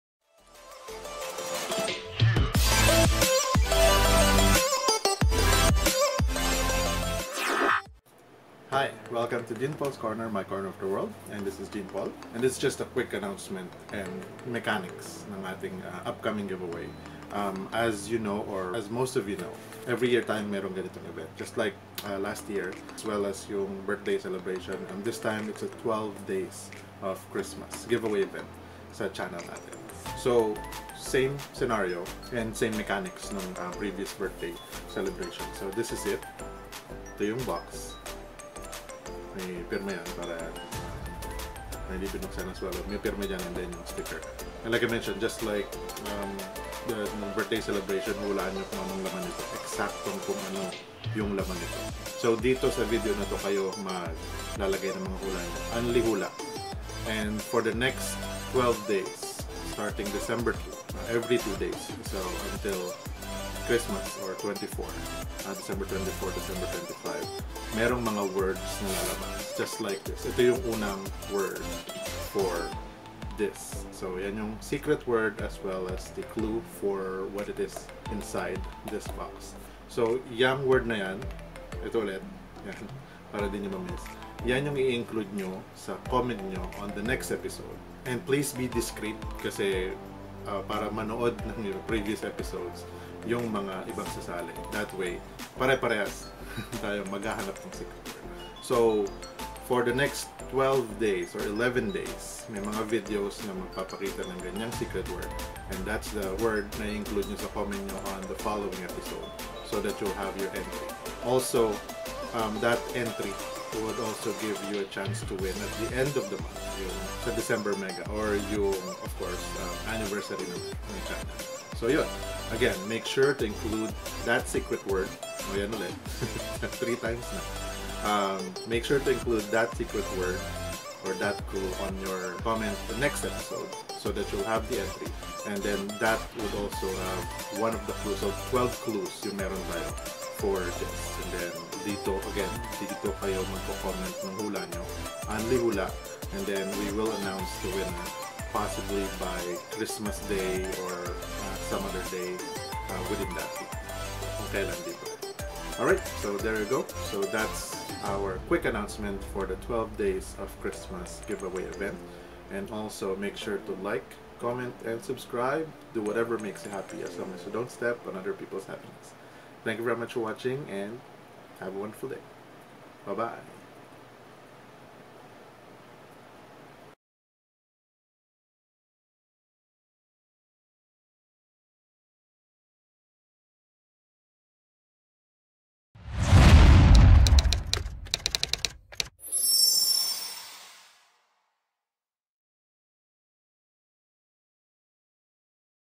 12 Days Of Christmas GIVEAWAY Announcement
Catch the secret words/clues and include them on your comments for the next and succeeding episodes to get your entry to the December MEGA, year-end and anniversary event. Also comment your guess of what the exact item is inside the box, and win it by the end of the event on or before December 25, Christmas day! Goodluck guys!

I hope you enjoy my uploads, so please...

Do whatever makes you HAPPY!!!